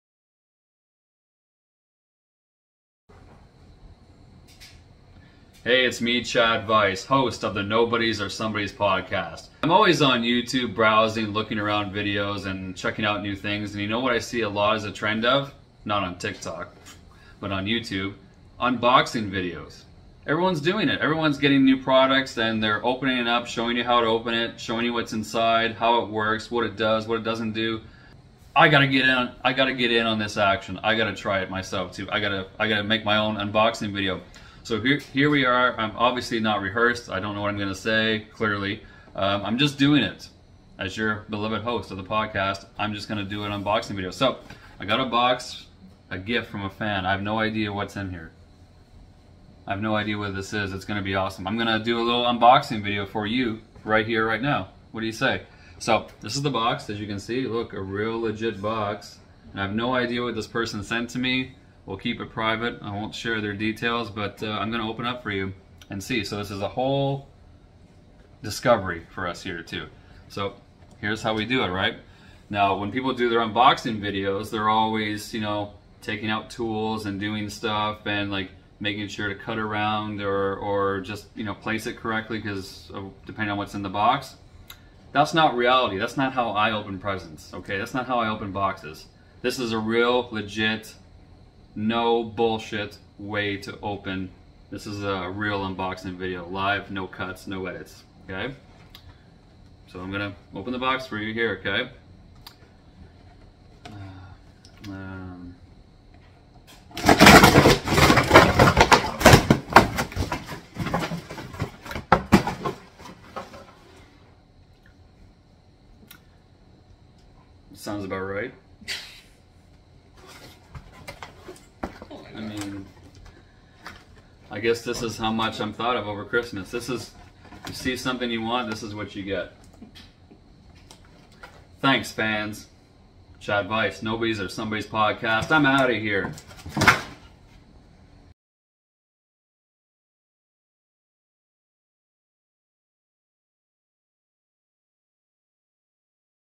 How To Unwrap a Complicated Package - Chad Vice Shows You The Way
Chad Vice does an unboxing video, and shows you the correct way to open a tricky box! What secret lies inside the box? Watch til the end and find out!